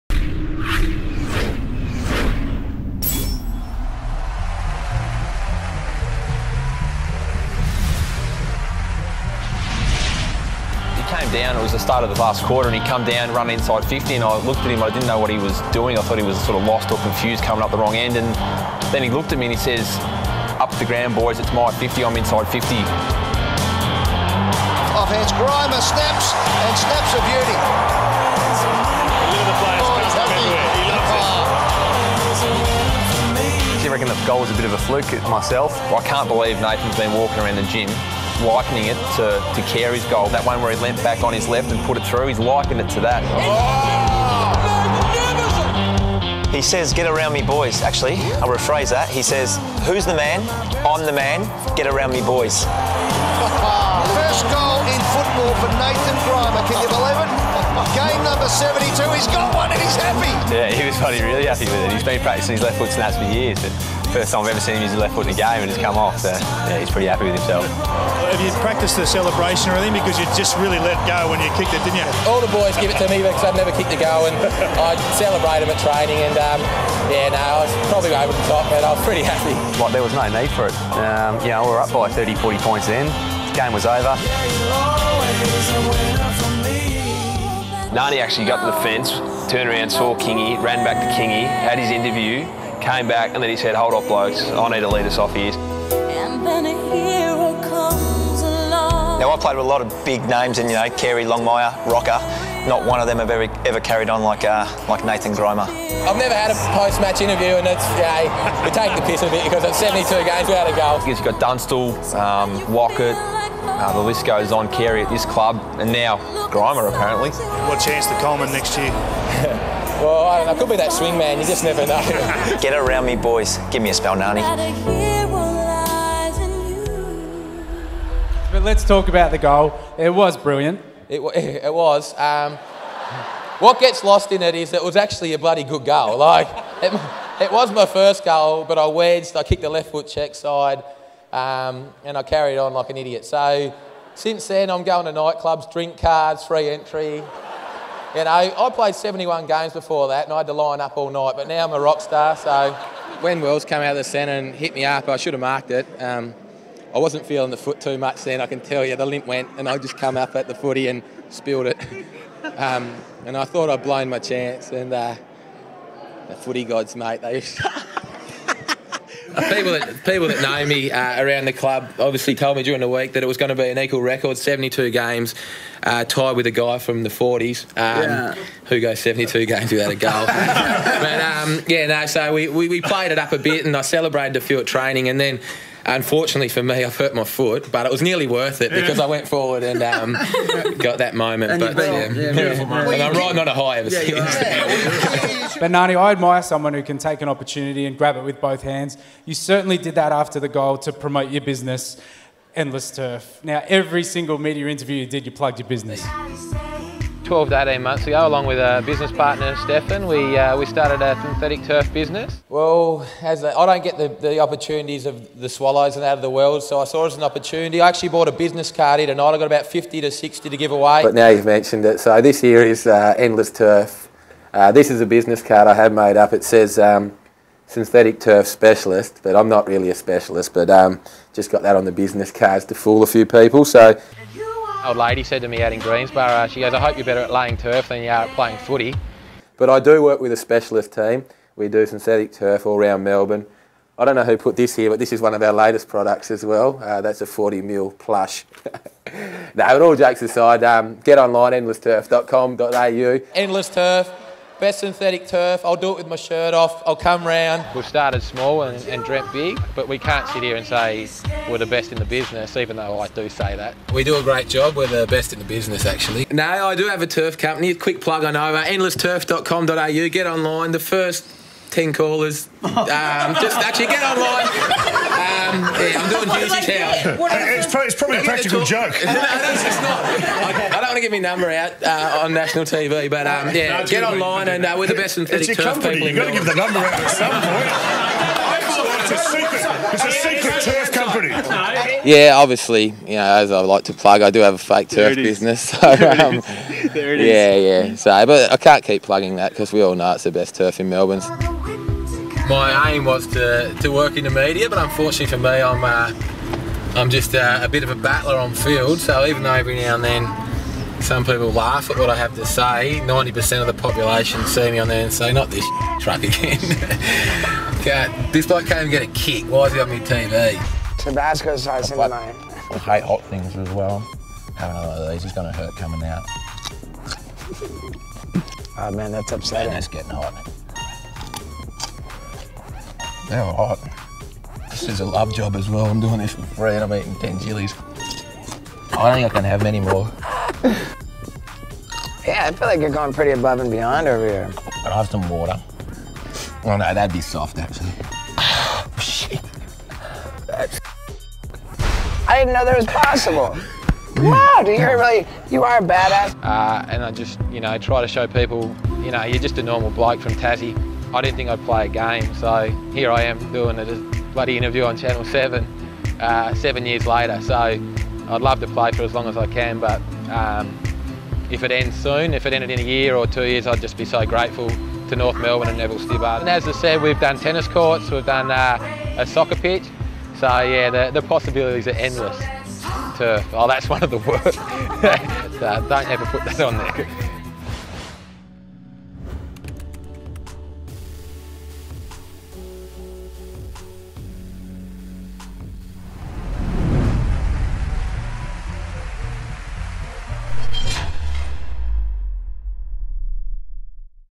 The best of Nathan Grima in the media
All the best media appearances from retired Roo Nathan Grima.